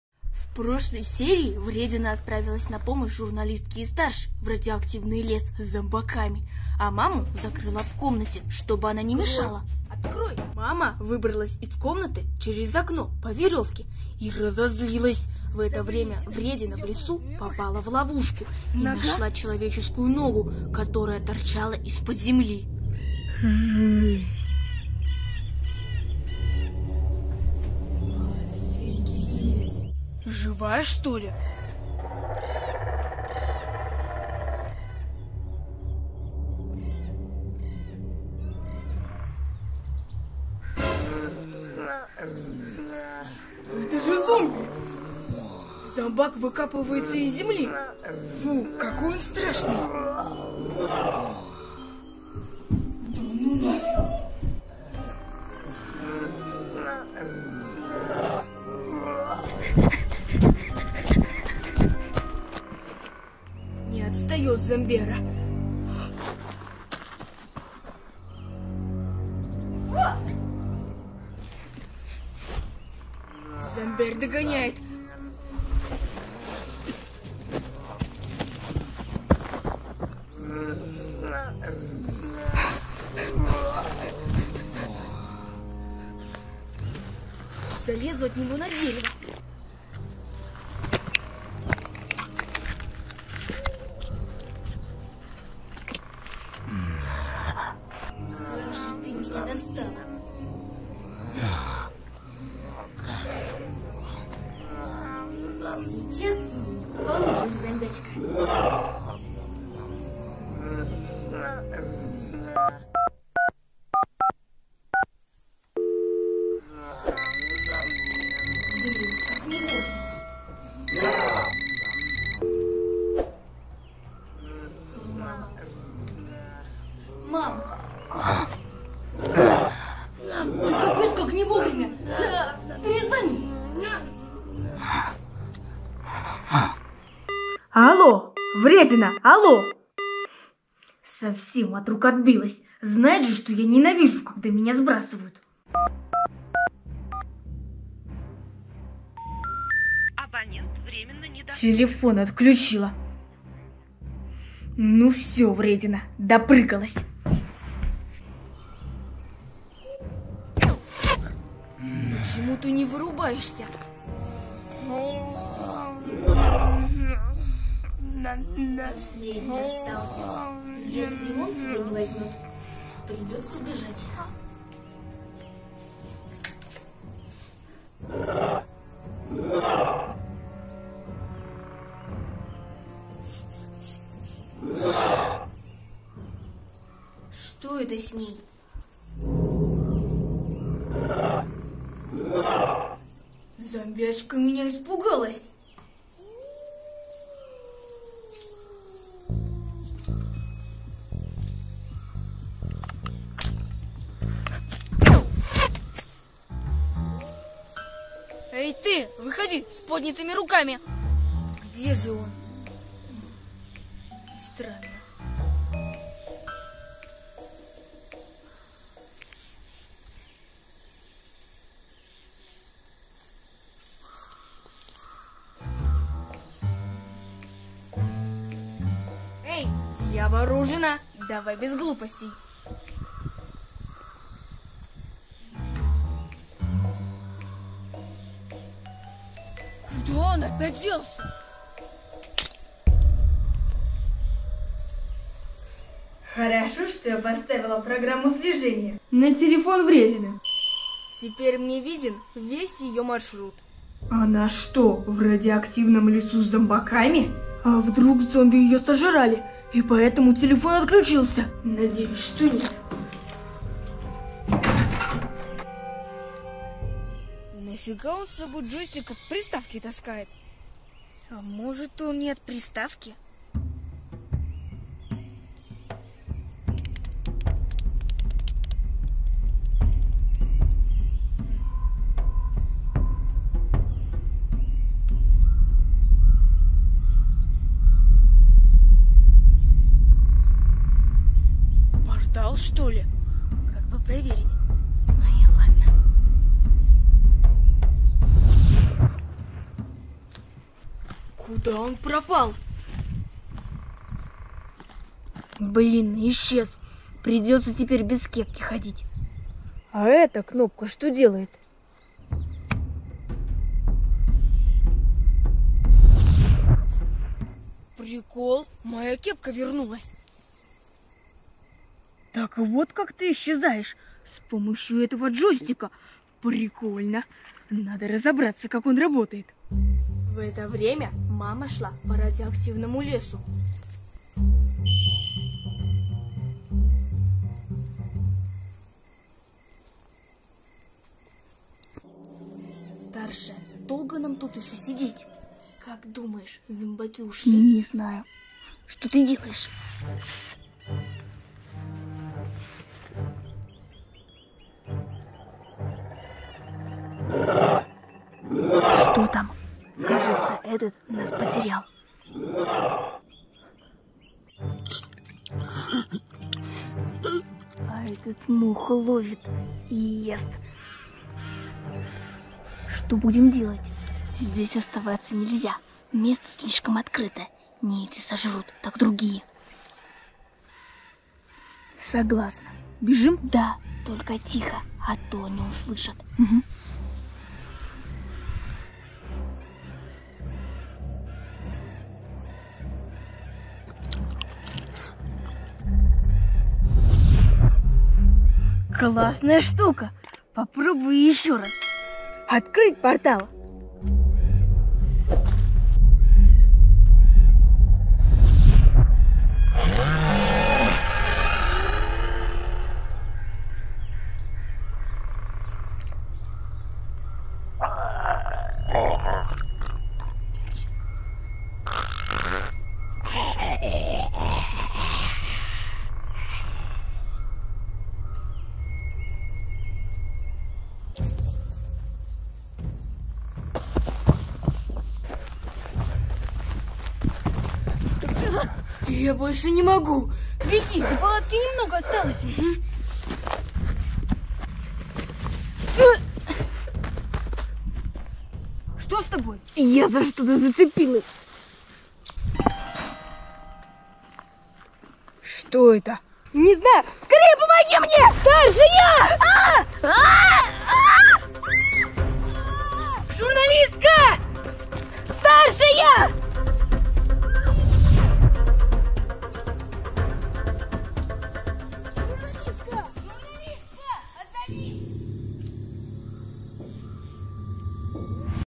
Сбежала от зомби. Вредина нашла портал в лесу. Серия 3
Зомби почти схватила Вредину, но девочке удалось сбежать. В радиоактивном лесу она нашла портал.